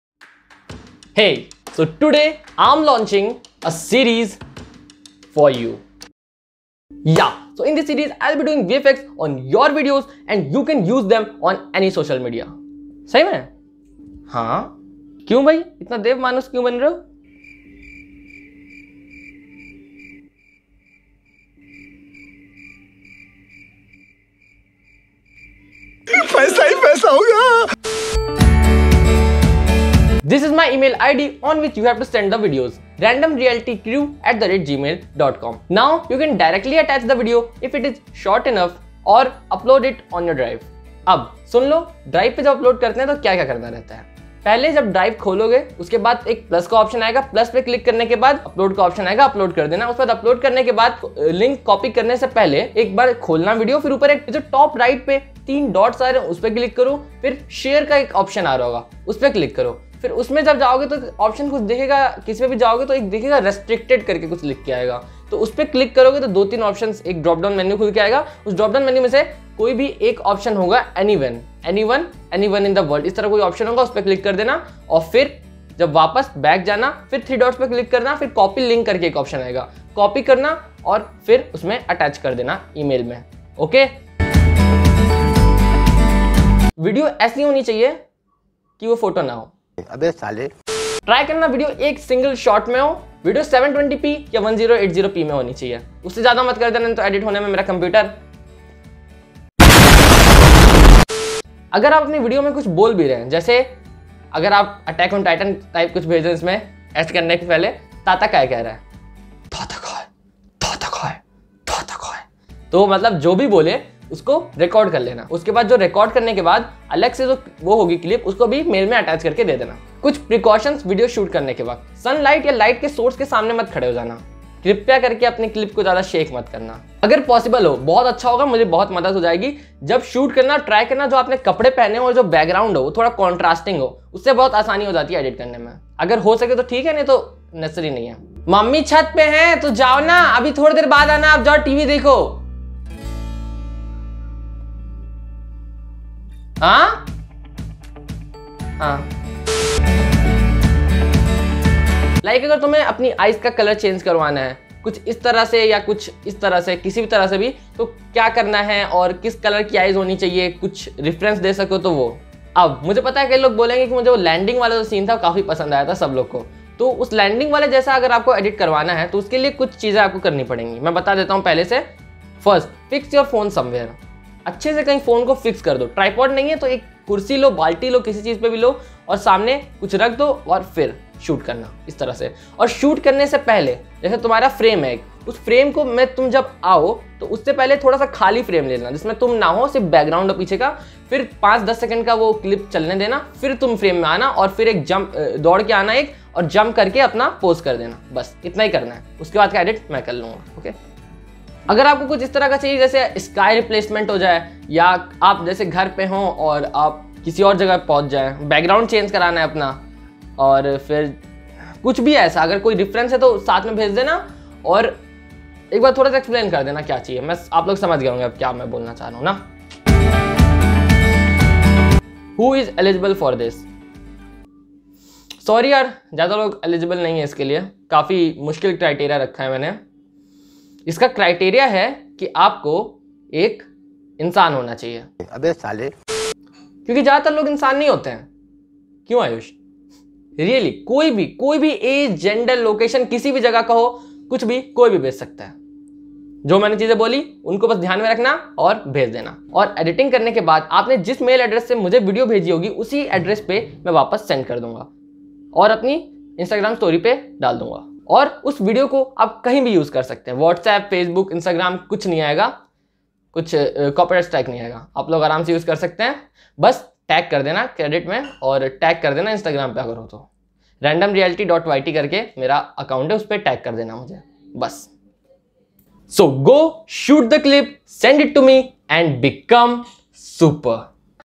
I'll do Vfx On Your Videos for Free - New Series Announcement
In this video I have announced a new series in which I will do vfx on your videos for free .
For more info checkout the video.

GO Shoot The Video , Send It To Me
         & BECOME SUPER